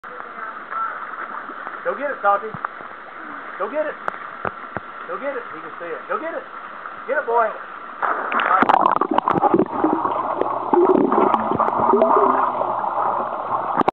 Salty Dog finally dives in!
Salty Dog after much hesitation of which angle is best to dive from, dives into the pool and underwater and shows off to the camera his techniques of retrieving his toys!

Be sure to check out Salty Dog's other great videos!!